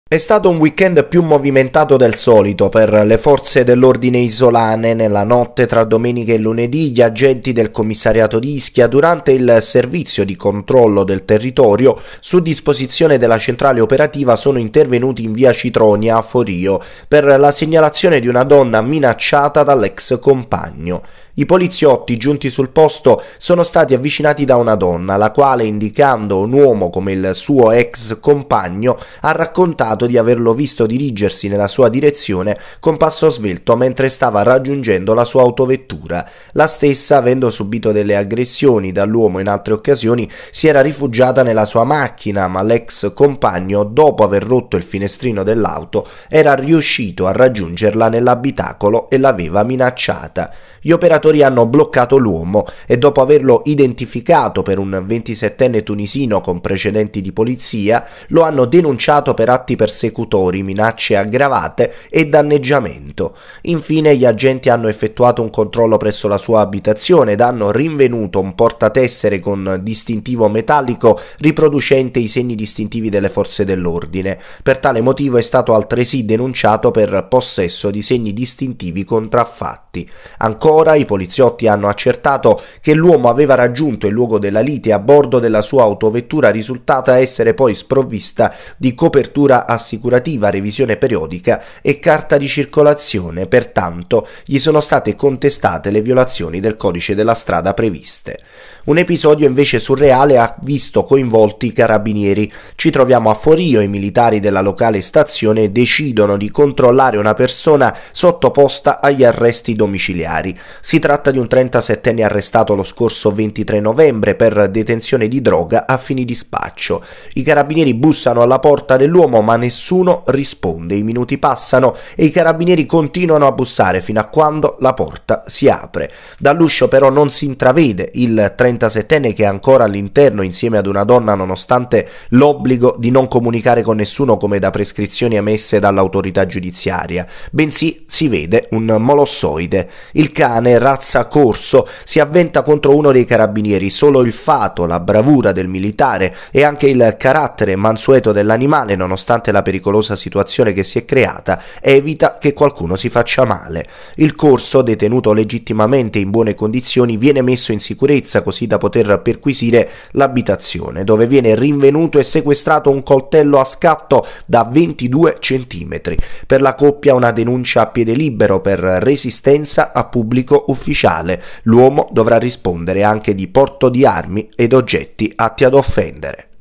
LA POLIZIA DENUNCIA TUNISINO PER MINACCE ALL’EX COMPAGNA.
CRONACA – LA POLIZIA DENUNCIA TUNISINO PER MINACCE ALL’EX COMPAGNA. CARABINIERI DEUNCIANO UOMO AGLI ARRESTI DOMICILIARI PER RESISTENZA A PUBBLICO UFFICIALE

Nel fine settimana due episodi di cronaca hanno riguardato sia la Polizia che i Carabinieri ed entrambi si sono verificati a Forio. Nel primo caso gli agenti del Commissariato di Ischia sono intervenuti su richiesta di una donna aggredita dall’ex compagno, nel secondo caso invece i Carabinieri hanno dovuto fare i conti con un cane Corso mentre controllavano un uomo sottoposto agli arresti domiciliari.